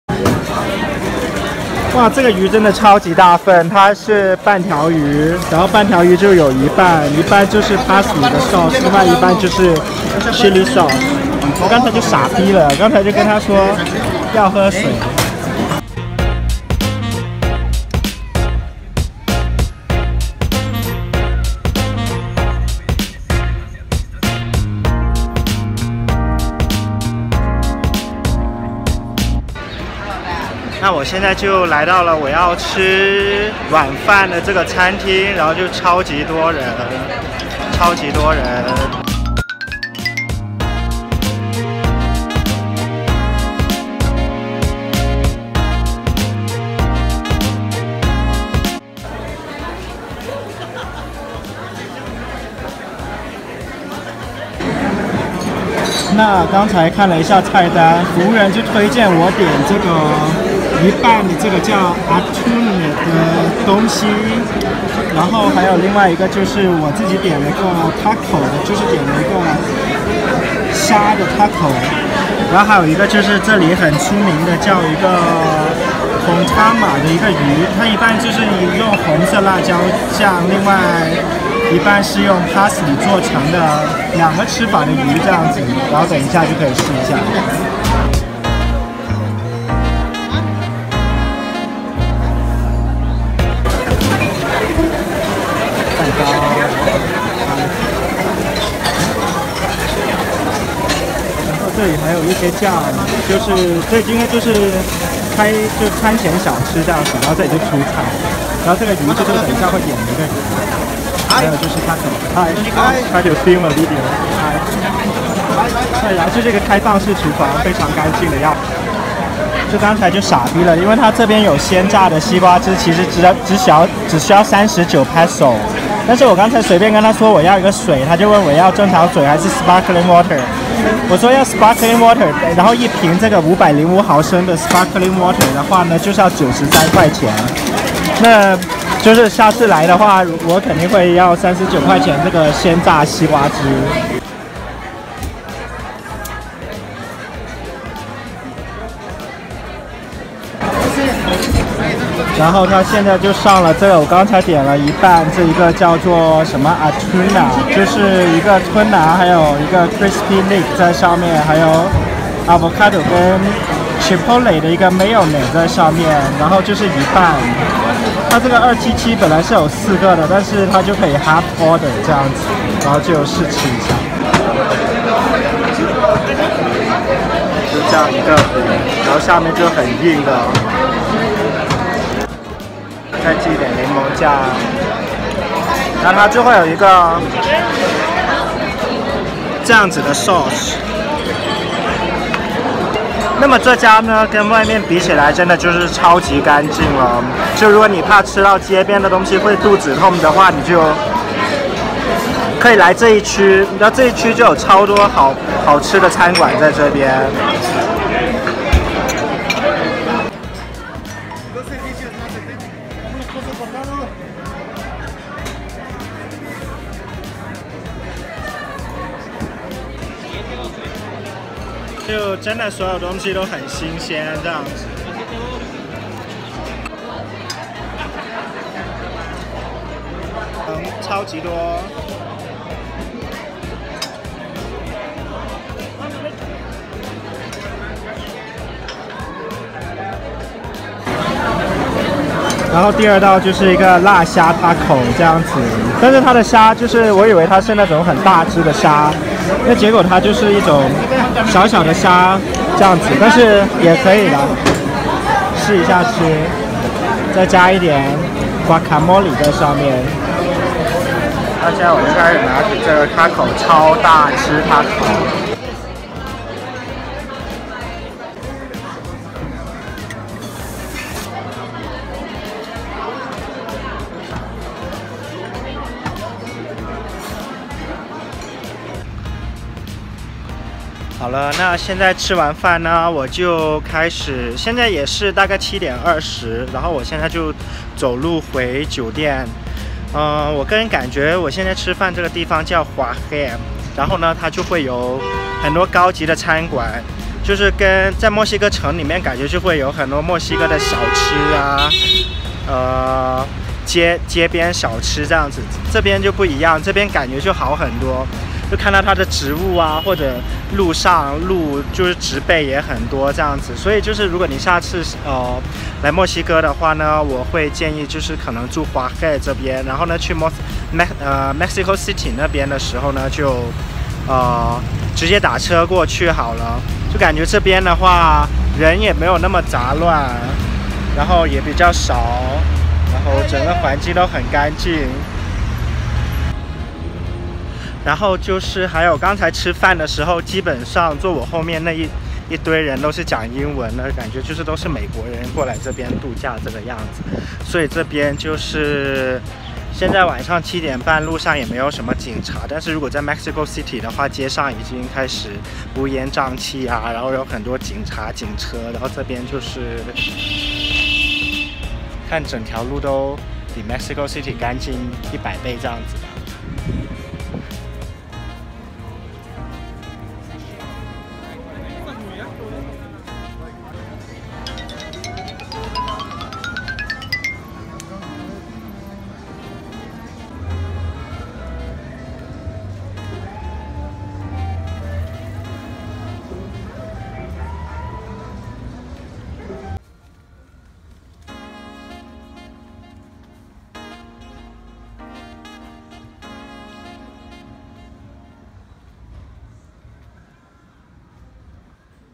第一次去墨西哥城应该住哪里？| 墨西哥城富人区吃顿饭人均？|juarez夜晚跟城里差很大！
逛完墨西哥城里的博物馆就打车去了富人区那边吃了个晚饭。 juarez算是比较富裕的城市在墨西哥城里边。 在这边比墨西哥城里安全太多。